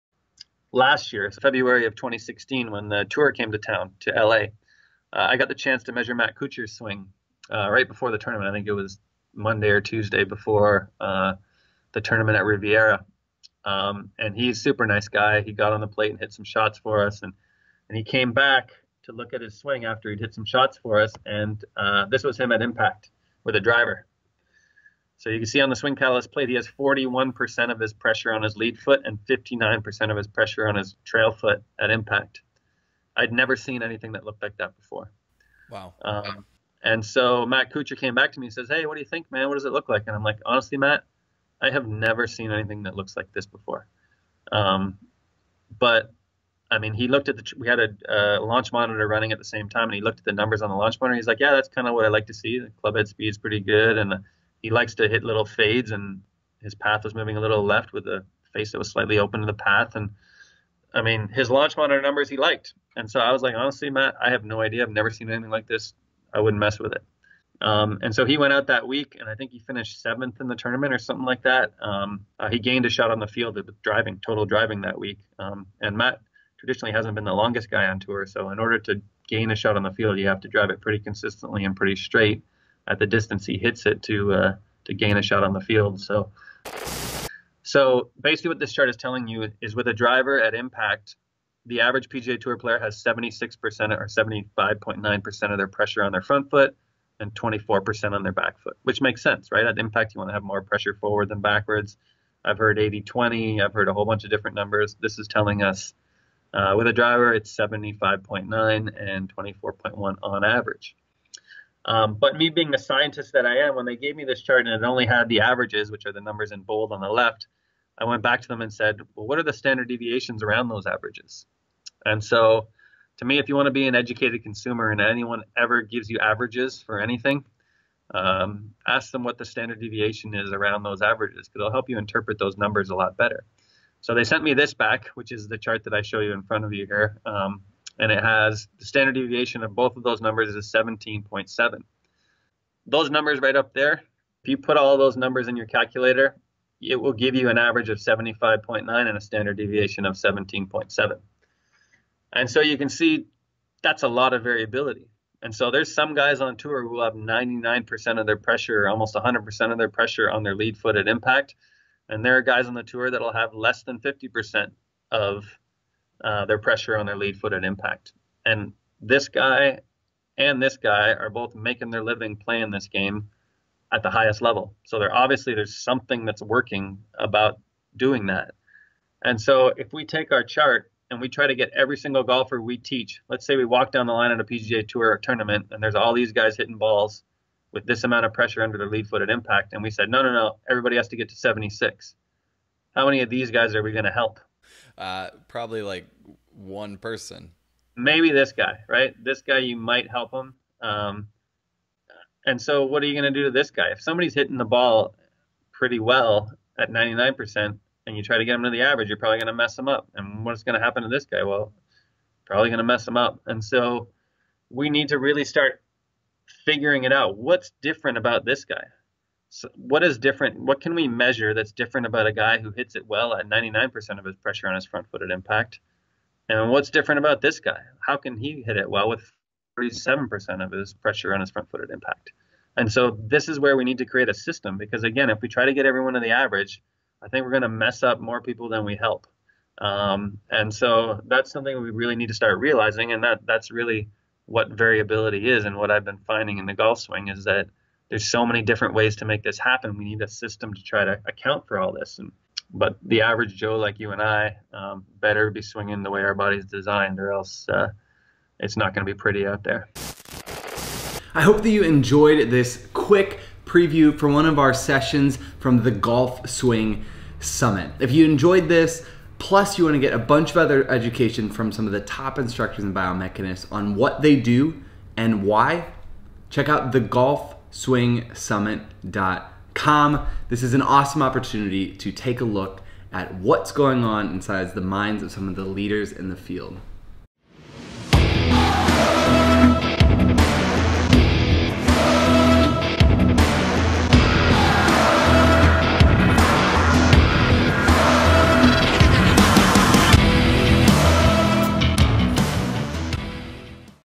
Making sense of VARIABILITY in the golf swing
Dr Scott Lynn shares his thoughts on variability in the golf swing and shares where he sees biomechanics helping golfers the most.